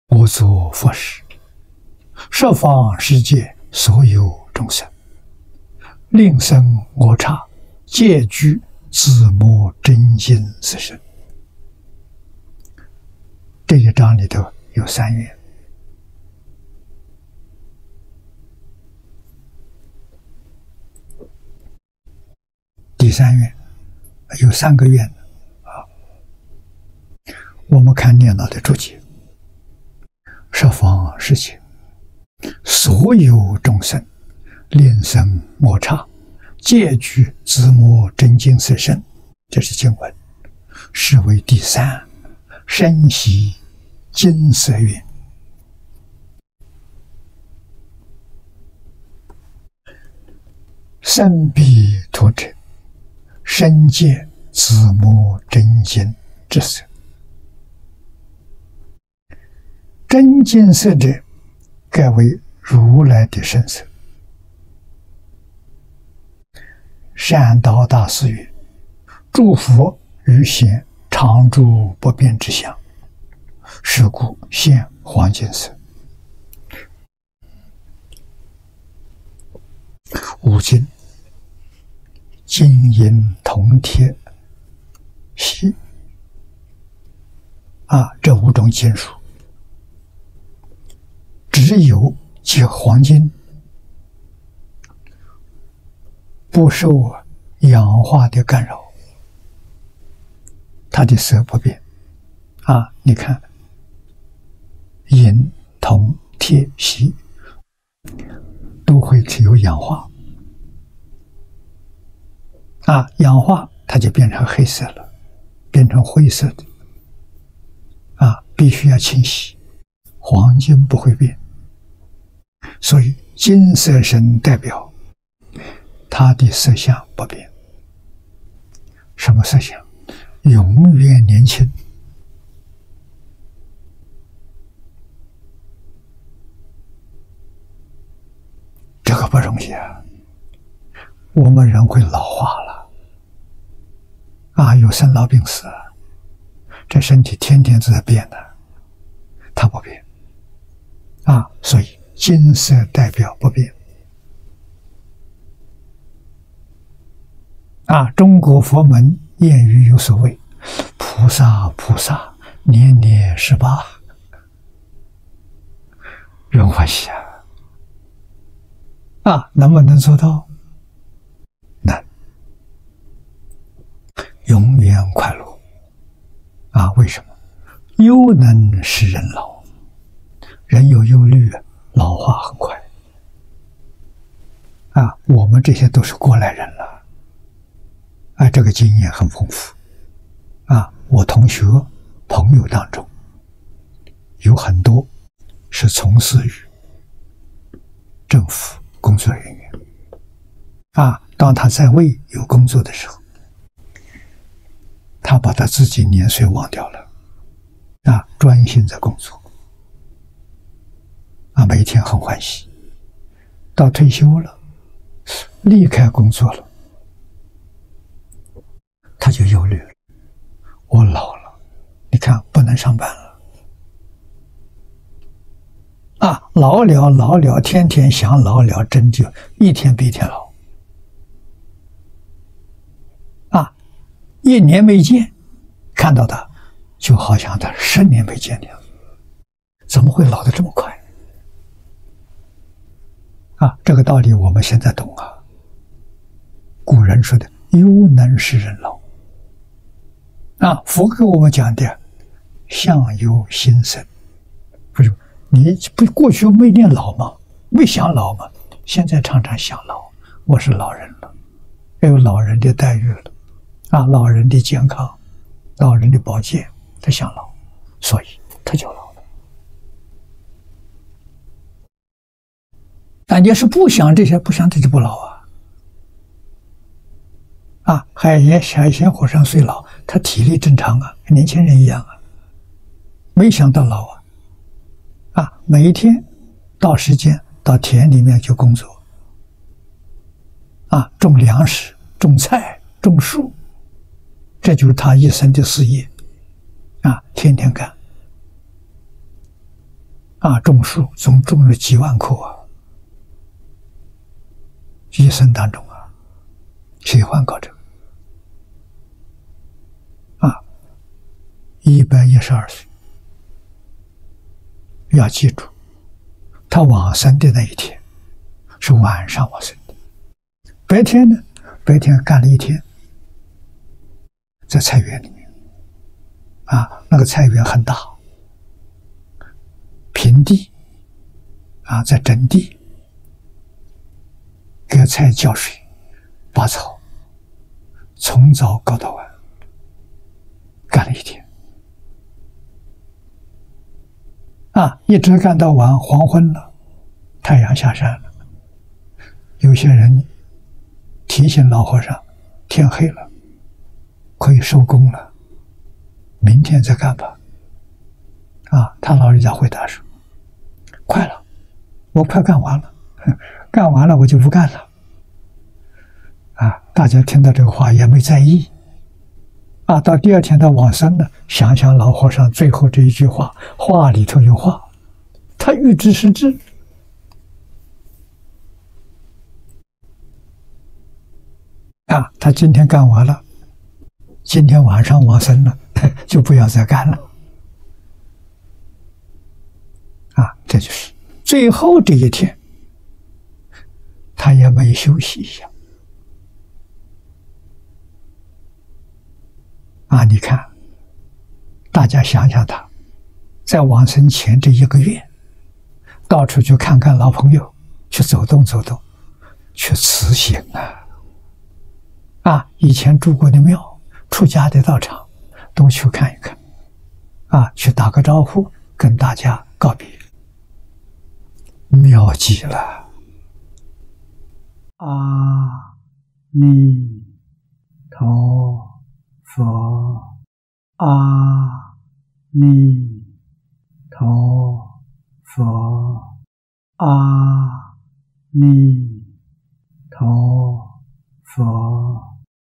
🙏淨空老法師🙏年年十八 能不能做到？🙏
【我作佛時。十方世界所有眾生。令生我剎皆具紫磨真金色身。】
　　這一章裡頭有三願，有三個願。我們看念老的註解，『十方世界所有眾生，令生我剎，皆具紫磨真金色身』，這是經文，「是為第三身悉金色願。生彼土者，身皆紫磨真金之色。真金色者，蓋為如來之身色。善導大師曰：諸佛欲顯常住不變之相，是故現黃金色。」五金，金銀銅鐵錫，這五種金屬，只有黃金不受氧化的干擾，它的色不變。你看，銀銅鐵錫都會氧化，氧化它就變成黑色、變成灰色的，必須要清洗，黃金不會變。所以金色身代表他的色相不變，什麼色相？永遠年輕。這個不容易，我們人會老化，有生老病死，這身體天天都在變，他不變，所以金色代表不變。中國佛門諺語有所謂「菩薩菩薩，年年十八」，人歡喜！能不能做到？能，永遠快樂。為什麼？憂能使人老，人有憂慮老化很快。我們這些都是過來人，這個經驗很豐富，我同學、朋友當中有很多是從事於政府工作人員，當他在位有工作的時候，他把他自己年歲忘掉了，專心在工作，每天很歡喜。到退休了，離開工作，他就憂慮，我老了，你看不能上班了。老了老了，天天想老了，真就一天比一天老，一年沒見，看到他就好像十年沒見的樣子，怎麼會老得這麼快？這個道理我們現在懂，古人說的「憂能使人老」，佛給我們講的「相由心生」，為什麼？你過去沒有念老、沒有想老，現在常常想老，我是老人，要有老人的待遇，老人的健康，老人的保健，他想老，所以他就老了。你要是不想這些，不想，他就不老。
海賢和尚雖老，他體力正常，跟年輕人一樣，沒想到老，每天到時間到田裡面去工作，種糧食、種菜、種樹，這就是他一生的事業，天天幹，種樹總種有幾萬棵，一生當中喜歡搞這個，一百一十二歲。要記住，他往生的那一天是晚上往生的，白天呢？白天幹了一天，在菜園裡面。那個菜園很大，平地，在整地，給菜澆水、拔草，從早幹到晚，幹了一天。一直幹到晚，黃昏了，太陽下山了，有些人提醒老和尚：天黑了，可以收工了，明天再幹！他老人家回答說：快了，我快幹完了，幹完我就不幹了。大家聽到這個話也沒有在意，到第二天他往生，想想老和尚最後這一句話，話裡頭有話，他預知時至，他今天幹完了，今天晚上往生，就不要再幹了。這就是最後這一天，他也沒有休息一下。你看，大家想想他，在往生前這一個月，到處去看看老朋友，去走動走動，去辭行！以前住過的廟，出家的道場，都去看一看，去打個招呼，跟大家告別，妙極了！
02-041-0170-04（年年十八 能不能做到？）《淨土大經科註（第四回）》(第170集)01:16:15-01:24:06
至誠感恩 ! 🙏❤🙏❤🙏❤